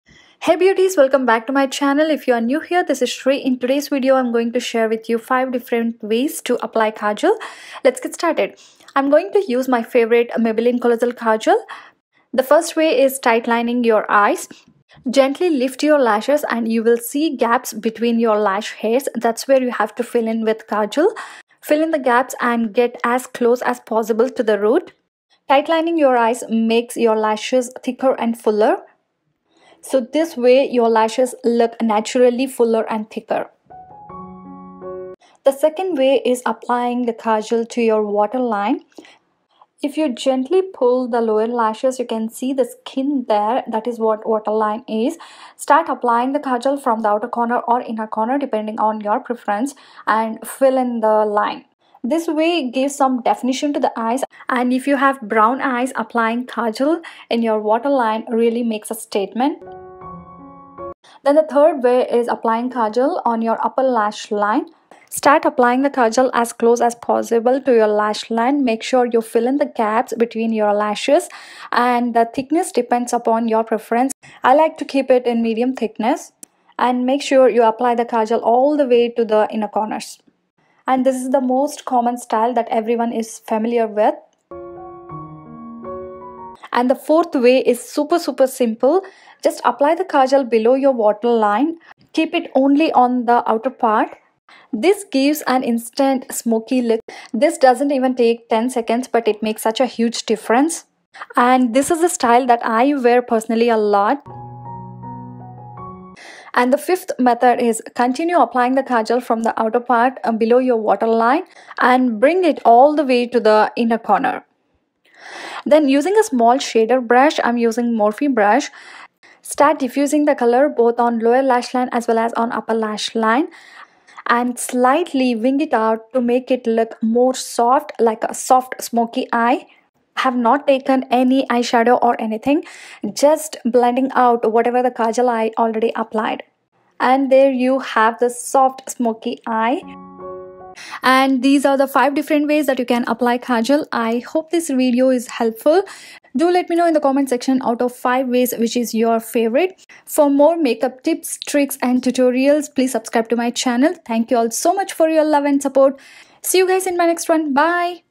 How To Apply Kajal In 5 Different Ways
Hello my beauties,
In today's video I have shared 5 different ways that you can apply Kajal! Each look takes less than a minute I promise and if you are a beginner then this is perfect for you:) For more Makeup tips, trick and tutorials, please subscribe to my channel:)

Product Used:
Maybelline colossal kajal in black

Editing software: Final Cut Pro